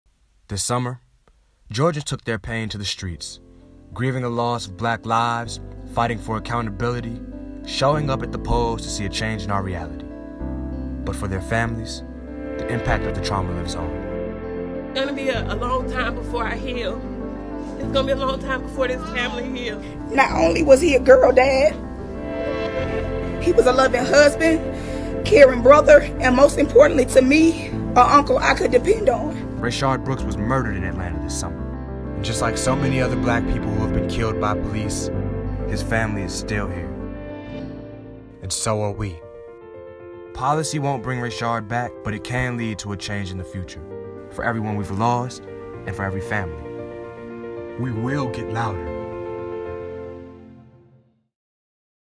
GetLouder for Georgia
Too many Black and Brown families have to live with the continued trauma of losing someone to police violence and it’s time we help amplify their voices.

Next week, early voting for the 2021 Senate runoff election begins in Georgia and we’re urging voters to show up in solidarity ahead of and on January 5th. It’s time we pass legislation to ensure a better future.